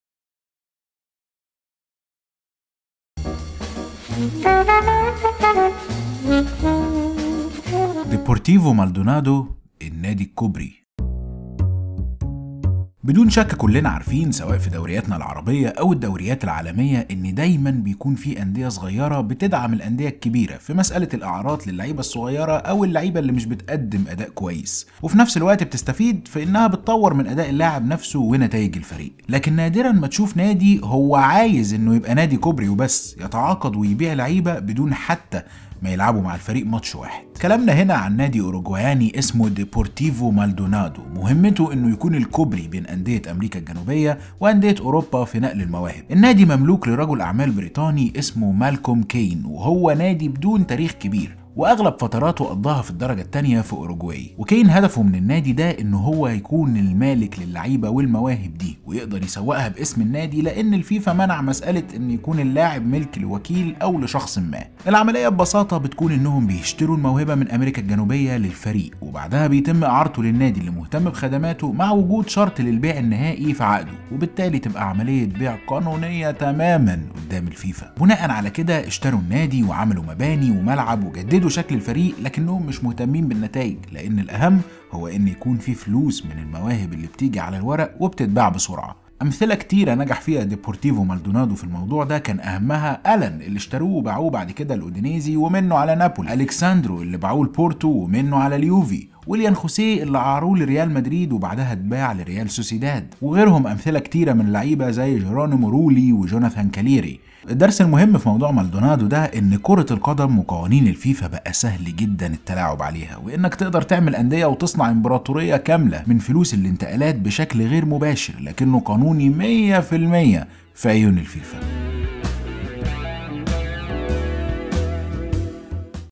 نادي الكوبري في كرة القدم
في هذه الحلقة من ورقة وقلم سنتحدث عن فكرة لا تستخدم كثيراً وهي نادي الكوبري في كرة القدم وهو ديبورتيفو مالدونادو.
من هو هذا النادي وكيف يعمل بين أندية امريكا الجنوبية وأوروبا وما هي وظيفته في سوق الانتقالات؟